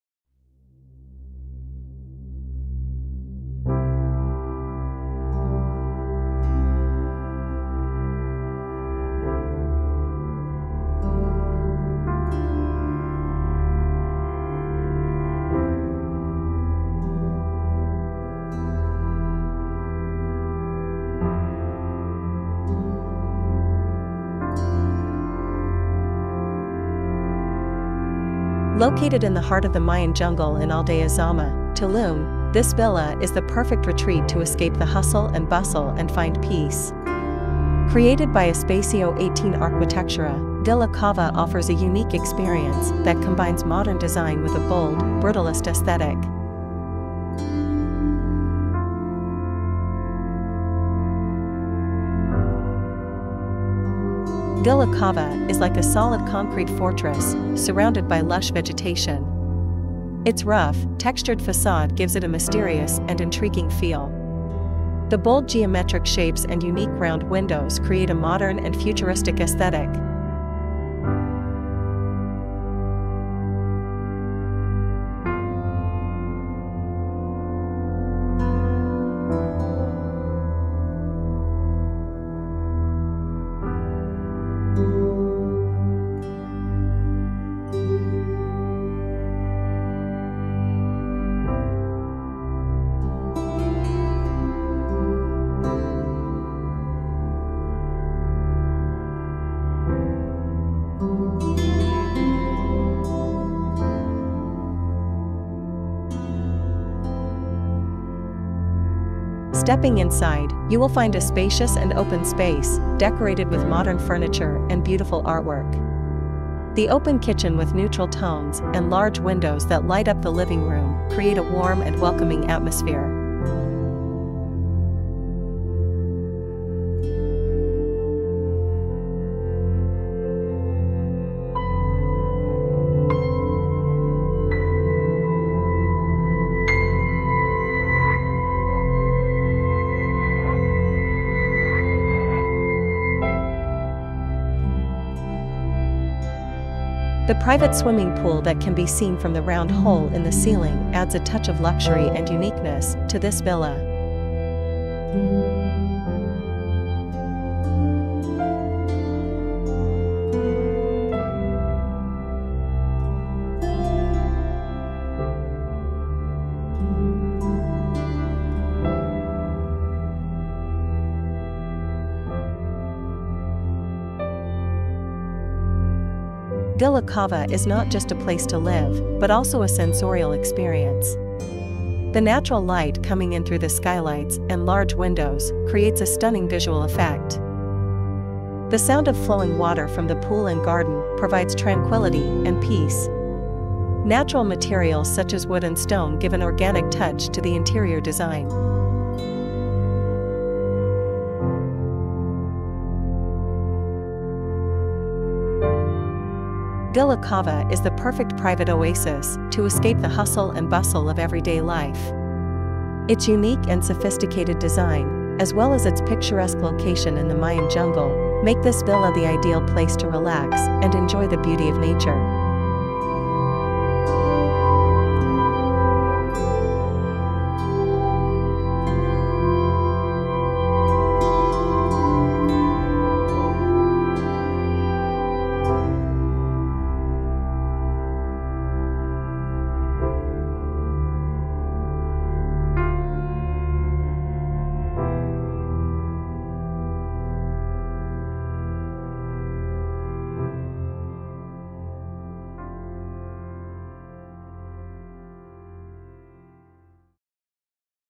The Stunning Design of a Cenote-Inspired Villa Featuring a Glass-Bottom Pool
Step into paradise with Villa Cava, a stunning architectural marvel nestled in the heart of Tulum's Mayan jungle. This video explores this unique villa, designed by Espacio18 Arquitectura in collaboration with a visionary couple from Canada.

Inspired by Nature, Designed for Experience

A Cenote Oasis: Discover the villa's captivating concept, resembling a natural cave with a breathtaking glass-bottom pool providing a skylight effect.
Luxury Through Nature: Witness how Villa Cava prioritizes connection with the surrounding jungle, offering a luxurious escape unlike any other.
Sustainable Design: Explore the use of local materials and construction techniques that harmonize with the environment and climate.
A Sculptural Journey

Carved from the Jungle: Unveil the villa's breathtaking exterior, a brutalist-inspired fortress resembling a carved sculpture.
Light and Water Spectacle: Be mesmerized by the use of natural light and water features, creating a multi-sensory experience throughout the villa.
Hidden Gems: Discover the secret entrance and the stunning lobby illuminated by the pool above.
Luxury Defined by Comfort

Live Like Royalty: Explore the expansive chef's kitchen, inviting social areas, and a swim-up pool, perfect for unforgettable gatherings.
A Sanctuary for Relaxation: Witness the private upper floor with bedrooms boasting soaring ceilings, natural light, and breathtaking jungle views.
Designed for Connection: See how Villa Cava caters to both social gatherings and individual privacy within its various spaces.
More Than Just a Villa

A Collaborative Masterpiece: Learn about the team effort behind Villa Cava, including local artisans and builders who brought this dream to life.
A Vision for the Future: Discover Adrian's vision to create a network of luxury villas that prioritize human experience and connection with nature.
Villa Cava is a testament to progressive design and a celebration of the natural world. Prepare to be inspired!

Project Name: Villa Cava
Architects: Espacio 18 Arquitectura
Area: 445 m²
Year: 2023
Photographs: César Béjar
Manufacturers: Cemex, Interceramic
Lead Architect: Caty and Pato
Interior Design: Kayla Pongrac
Development: Adrian Salamunovic
Landscape: Di Tulum
Team: Mario Ávila, Carla Osorio, Sonia Morales, Andrea Fox, Adrian Salamunovic, Caty, Pato and Kayla Pongrac
City: Tulum
Country: Mexico